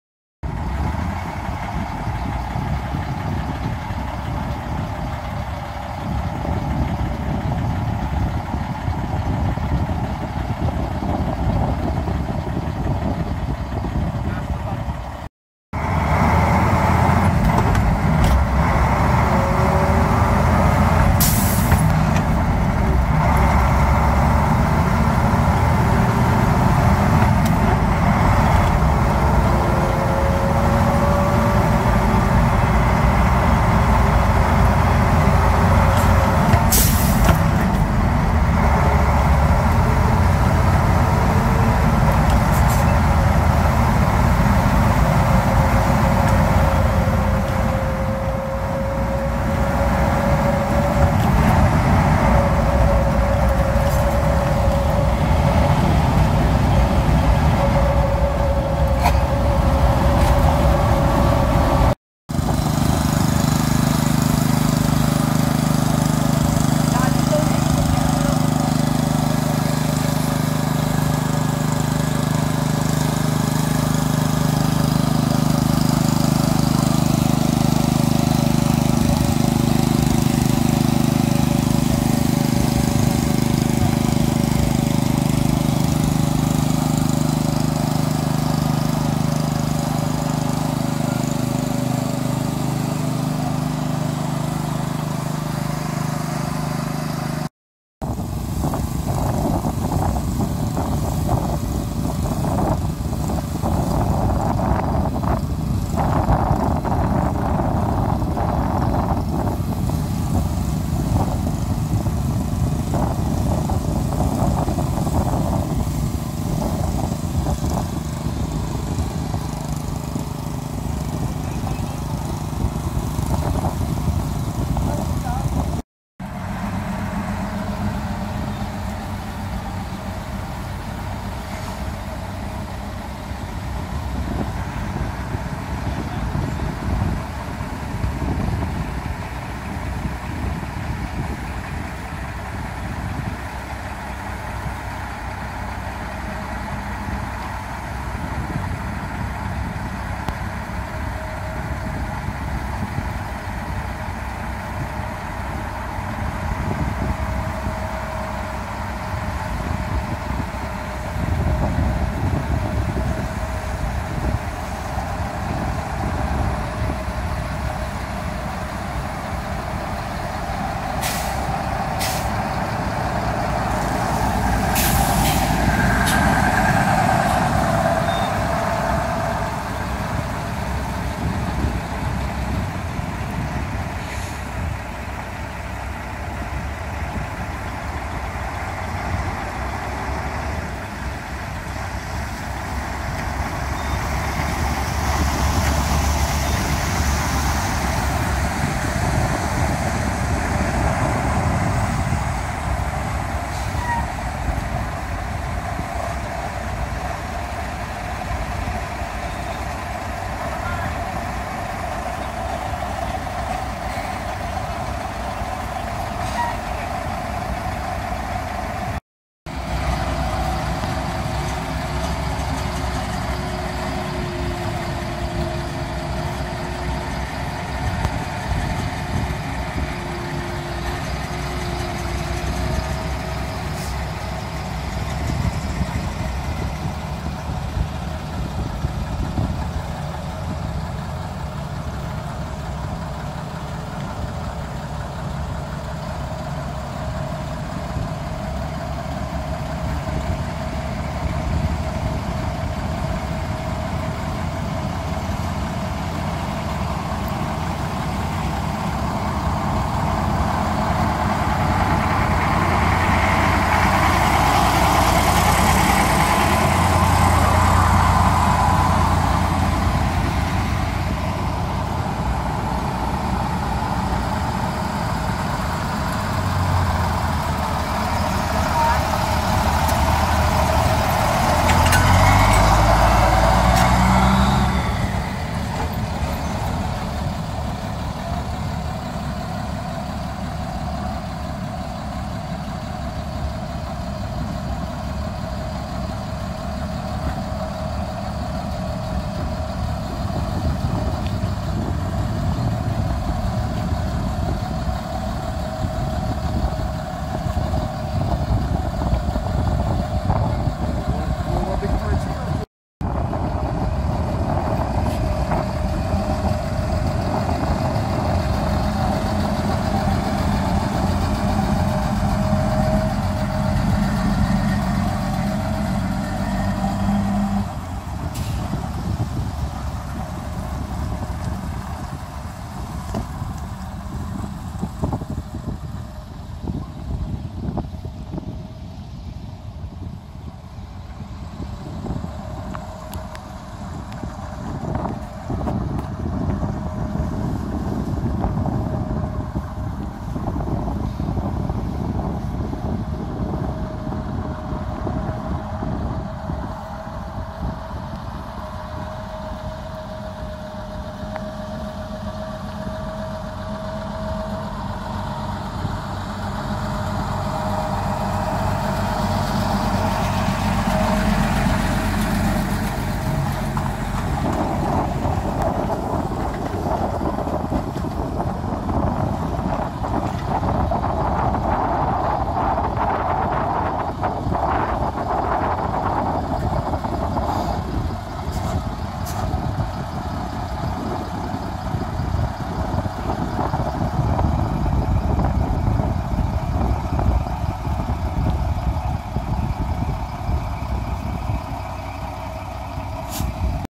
Staré náklaďáky na natáčení / Old trucks on filming
Kdysi dávno při přípravě na jedno natáčení.

Liaz 100.47
Volvo F88
Škoda 706 RTTN
Liaz 110
Tatra 148